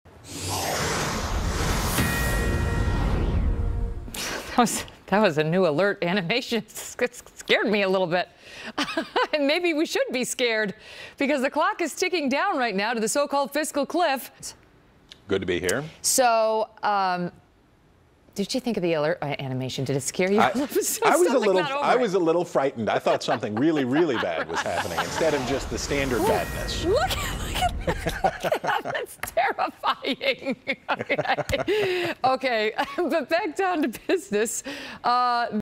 Megyn Kelly is Scared of Fox News Graphics
The new graphics are scaring Megyn Kelly.